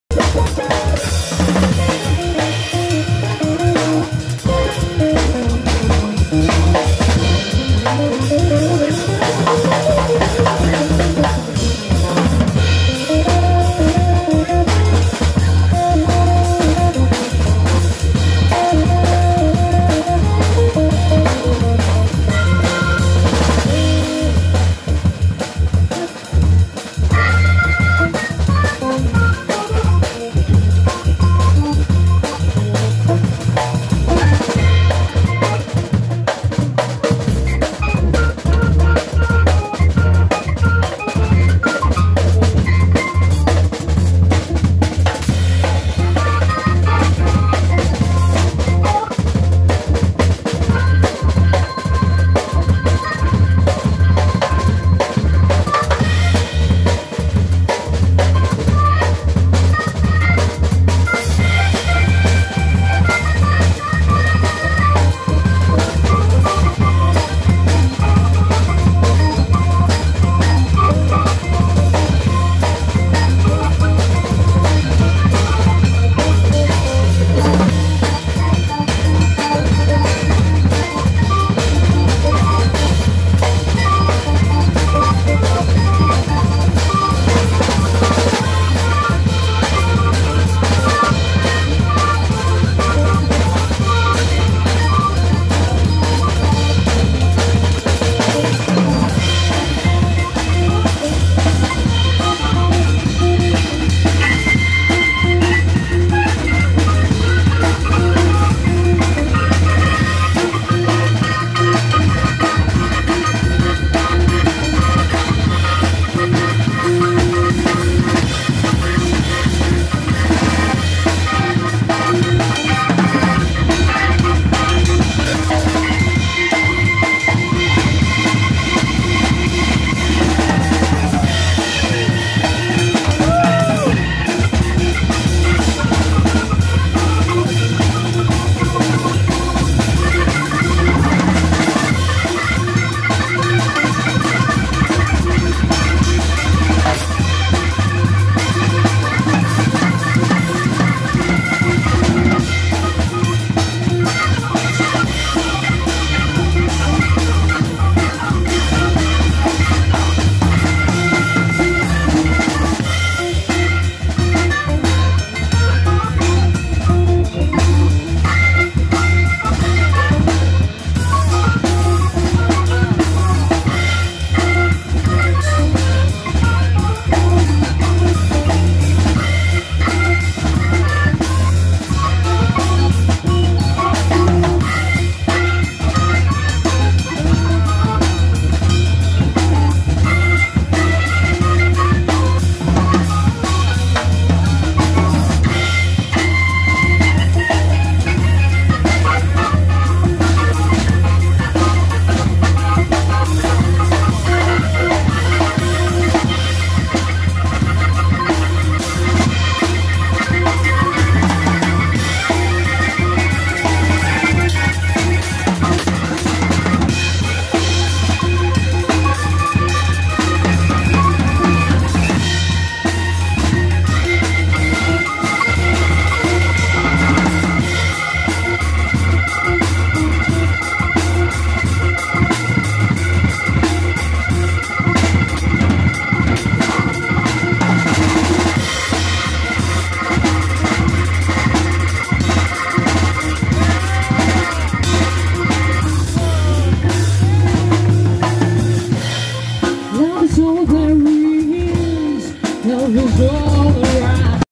Lela Kaplowitz Quartet- live in Vukovar- Rich Bar
Vukovar-  Rich Bar 8.8.2021
Lela Kaplowitz -voc
Joe Kaplowitz- organ
Dado Marinkovic- drums
Bruno Micetic -guit